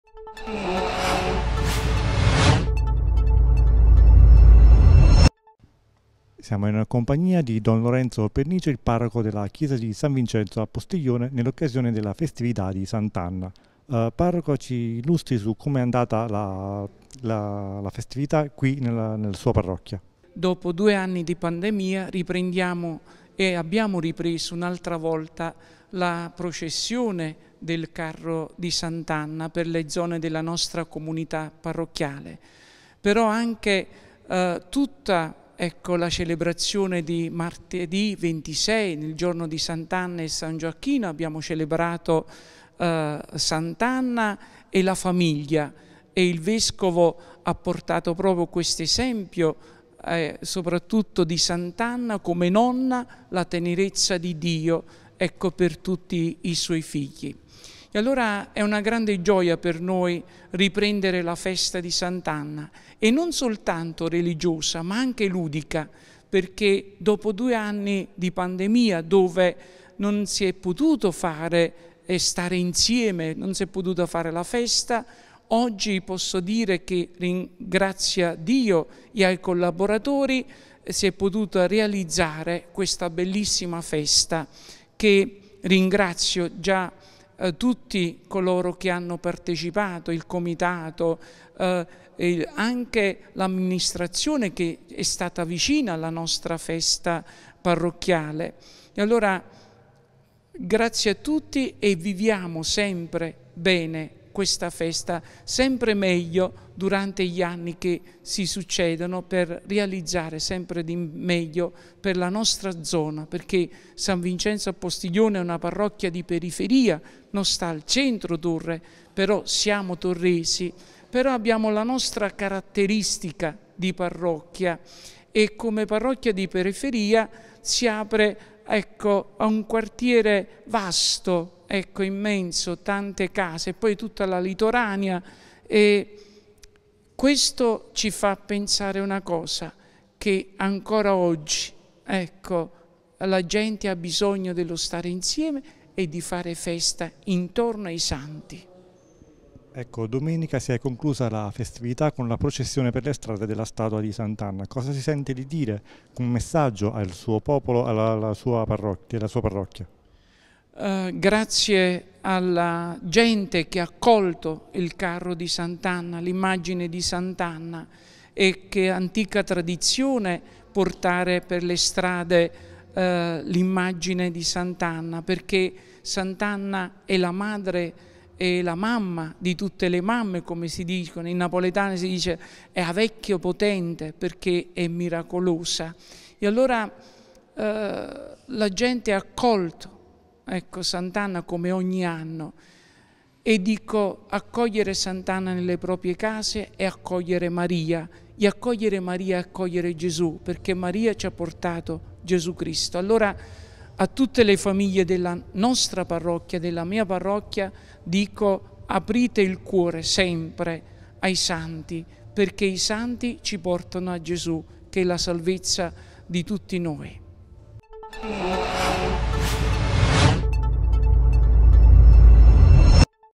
Festa di S.Anna a Torre del Greco. Intervista Don Lorenzo Pernice.
Festa di S.Anna a Torre del Greco presso la Chiesa di S.Vincenzo a Postiglione, con la processione in strada della statua di S.Anna.

Intervista Don Lorenzo Pernice.